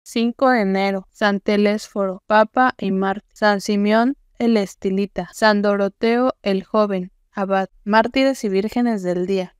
5 de enero, Mártires y Vírgenes del día
San Telésforo Papa y Mártir, San Simeón el Estilita, San Doroteo el Joven Abad